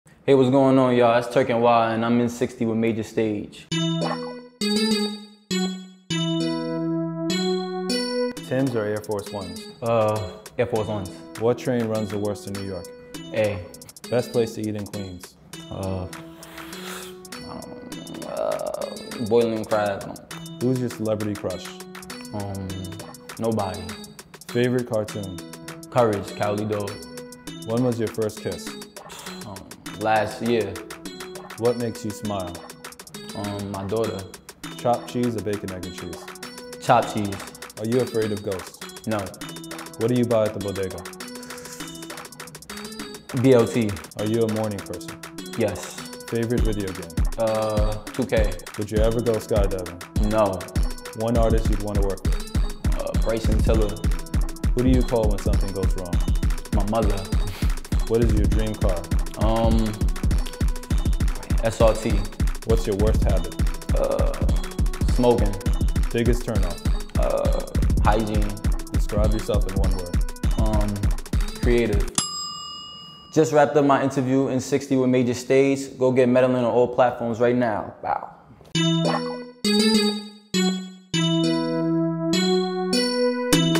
Turk Ny's first kiss was last year | in60 Interview
@turkny answers rapid fire questions in our in60 interview series at MajorStage studios. He tells us his favorite cartoon and the worst train in Ny.